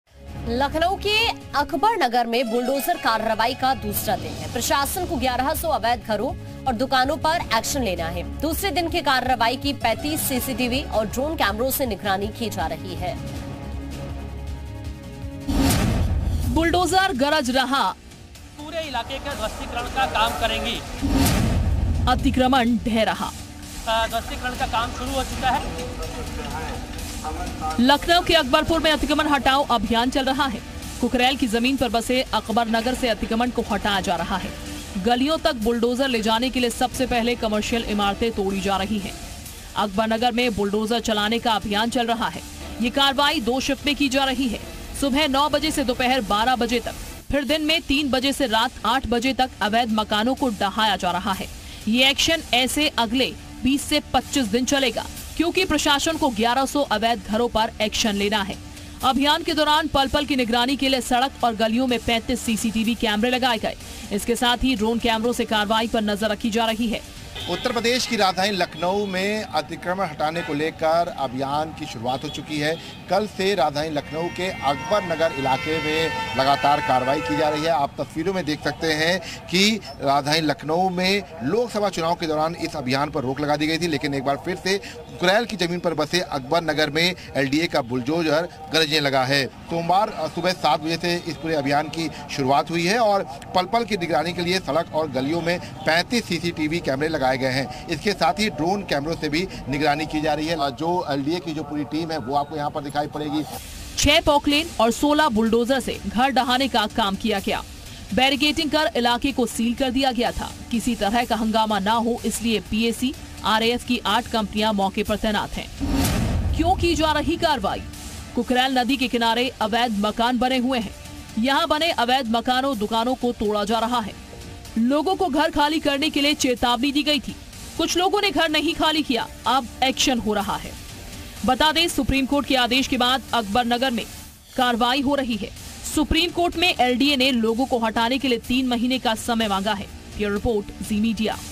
Bulldozer Action: Akabarnagar में जारी है Bulldozer Action, ड्रोन कैमरों की निगरानी में कार्रवाई |UP
Khabar Uttar Pradesh : अकबरनगर में जारी है बुलडोजर एक्शन, CCTV, ड्रोन कैमरों की निगरानी में कार्रवाई, 1100 मकान-दुकानों पर लिया जाना है एक्शन

Bulldozer action continues in Akbarnagar, action is being taken under the surveillance of CCTV, drone cameras, action is to be taken on 1100 houses and shops.